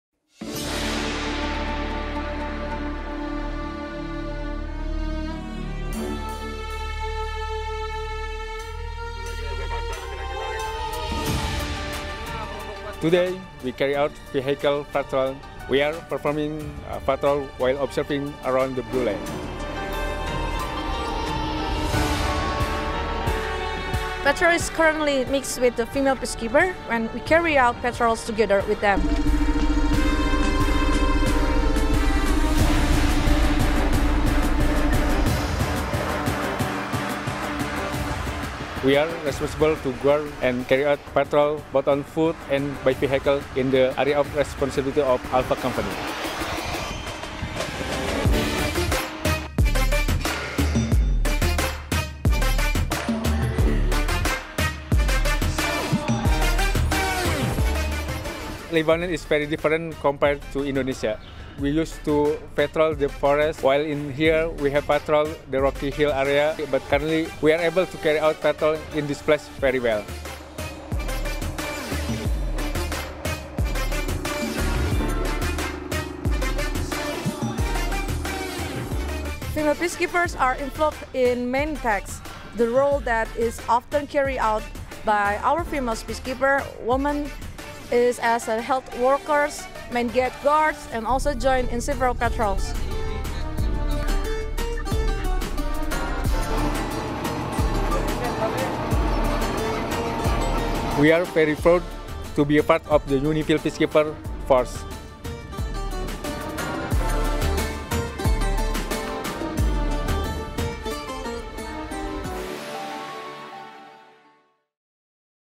​​​​​​​Gagahnya Arjuna dan Srikandi Penjaga Perdamaian Indonesia di UNIFIL (Dengan Terjemahan)
Tidak hanya menghalangi Tank, Pasukan Penjaga Perdamaian Indonesia di UNIFIL memberikan kontribusi yang besar dalam menjaga perdamaian di Garis Biru / Blue Line Lebanon. Gagah nya Arjuna dan Srikandi Indonesia ini diakui oleh seluruh bangsa. Indonesia saat ini adalah kontributor terbesar penjaga perdamaian dunia di UNIFIL dengan 1.300 personil dan ke 8 terbesar di dunia dengan lebih dari 3.500 personil. Yuk simak Video yang dibuat oleh UNIFIL bekerja sama dengan Pasukan Penjaga Perdamaian Indonesia.

Jakarta, Indonesia